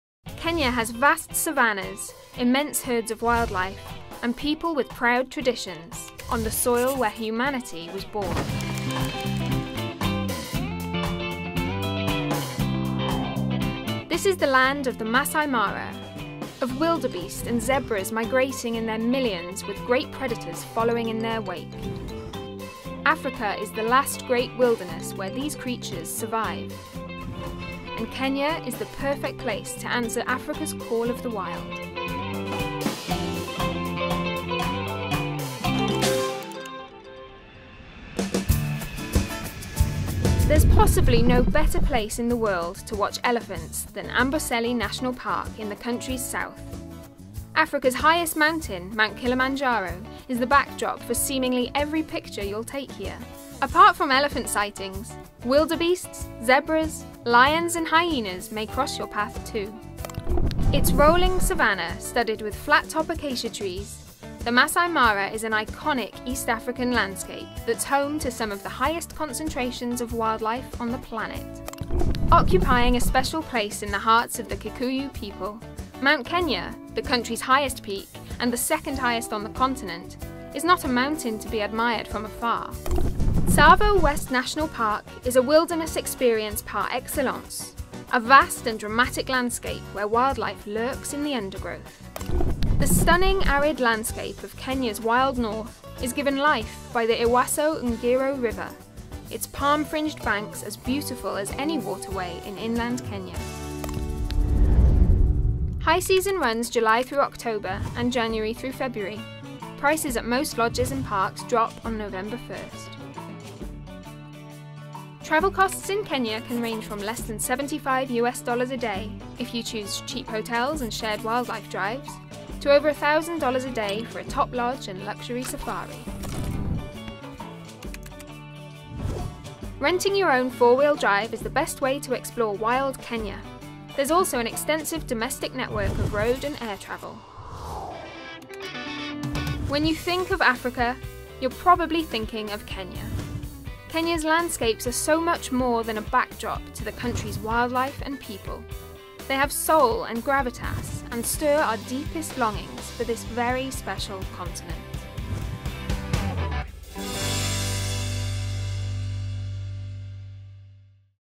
Introducing Kenya
Start exploring Kenya with Lonely Planet’s video guide to getting around, when to go and the top things to do while you're there.